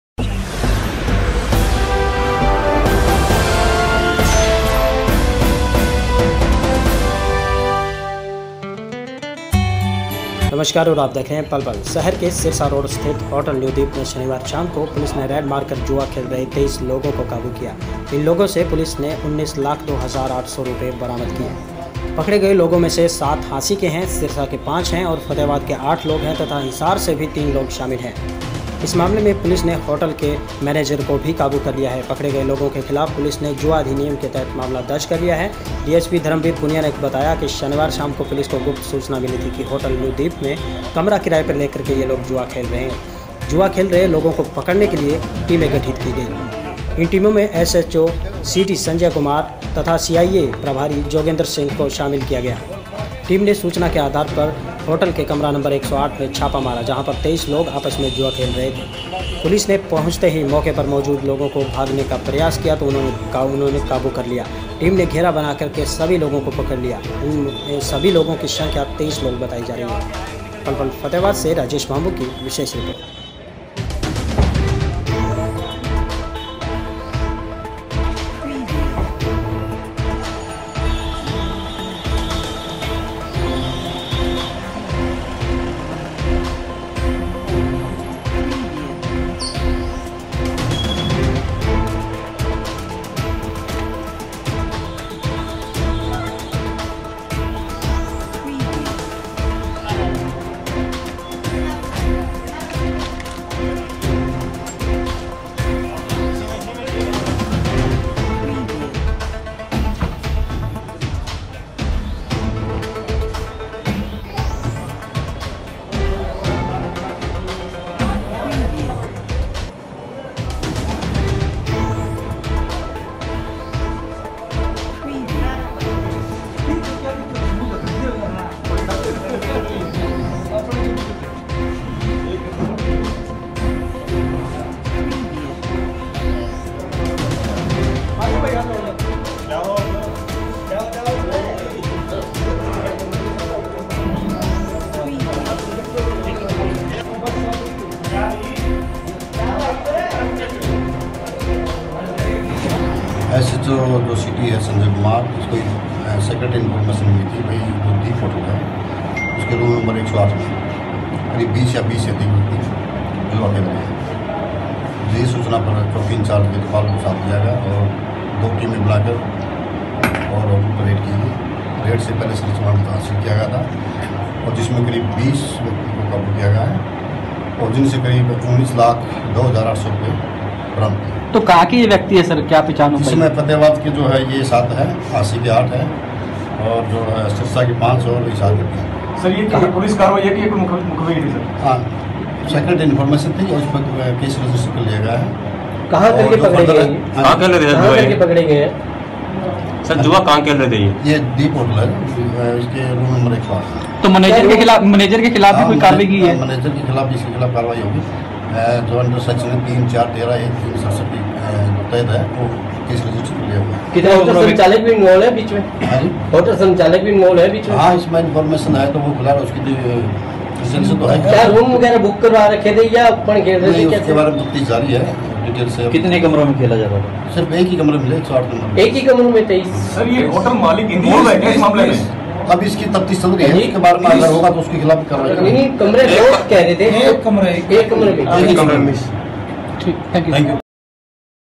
स्थित होटल में शनिवार को पुलिस ने रेड मारकर जुआ खेल रहे 23 लोगों को काबू किया
शहर के सिरसा रोड स्थित होटल में शनिवार को पुलिस ने रेड मारकर जुआ खेल रहे 23 लोगों को काबू किया
फ़तेहाबाद-शहर के सिरसा रोड स्थित होटल न्यू दीप में शनिवार शाम को पुलिस ने रेड मारकर  जुआ खेल रहे 23 लोगों को काबू किया है। इन लोगों से पुलिस ने 19 लाख 2 हजार 800 रुपये जुआ राशि बरामद की है। पकड़े लोगों में हांसी के 7, सिरसा के 5, फतेहाबाद के 8 तथा हिसार के 3 लोग शामिल हैं। इस मामले में पुलिस ने होटल के मैनेजर को भी काबू कर लिया है। पकड़े गए लोगों के खिलाफ पुलिस ने जुआ अधिनियम के तहत मामला दर्ज किया है।  डीएसपी धर्मबीर
पूनिया ने बताया कि शनिवार शाम को पुलिस को गुप्त सूचना मिली कि होटल न्यू दीप में कमरा किराए पर लेकर कुछ लोग जुआ खेल रहे हैं। जुआ खेल रहे लोगों को पकड़ने के लिए टीम का गठन किया गया। जिसमें एसएचओ सिटी संजय कुमार, सीआइए प्रभारी जोगेंद्र को शामिल किया गया। टीम ने सूचना के आधार पर होटल के कमरा नंबर 108 में छापा मारा। यहां पर 23 लोग आपस में जुआ खेल रहे थे। पुलिस के पहुंचते ही मौके पर मौजूद लोगों ने भागने का प्रयास किया। टीम ने घेरा बनाकर सभी को पकड़ लिया। मौके से 23 लोगों को काबू किया गया।